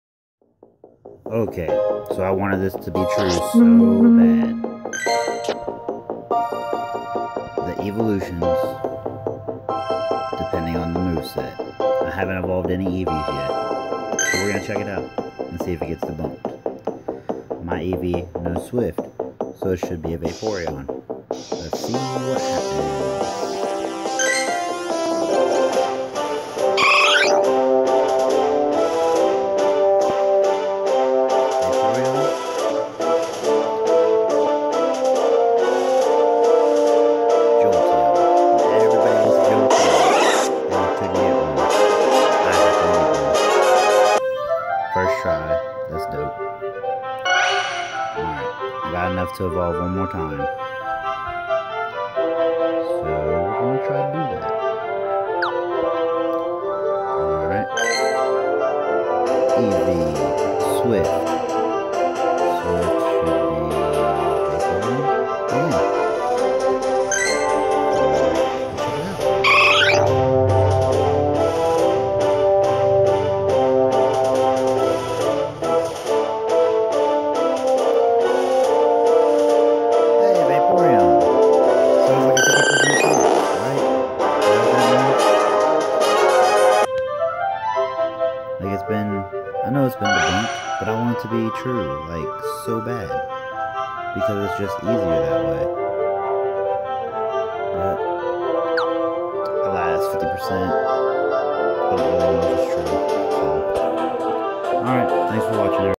Eevolutions myth debunked
Did it twice, 50% success rate so yeah debunked. I wanted to believe so bad. Pardon the commentary, isn't really loud ill tweak it for future videos.